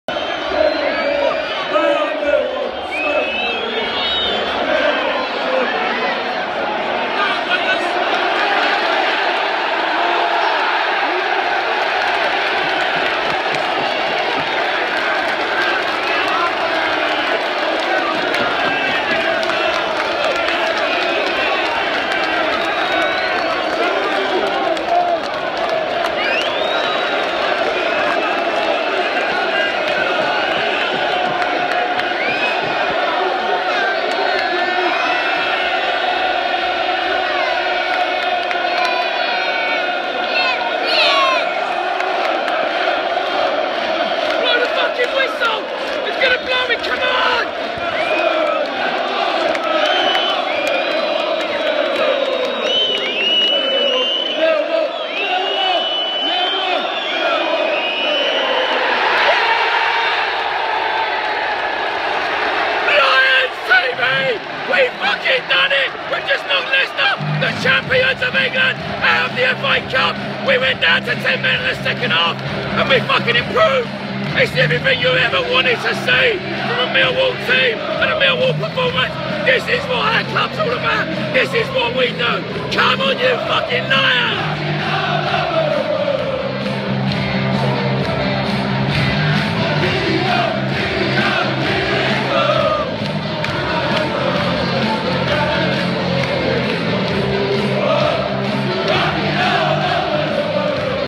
Millwall 1-0 Leicester - FULL TIME REACTION
FULL TIME REACTION from the den as we knock leicester the champions of england out of the fa cup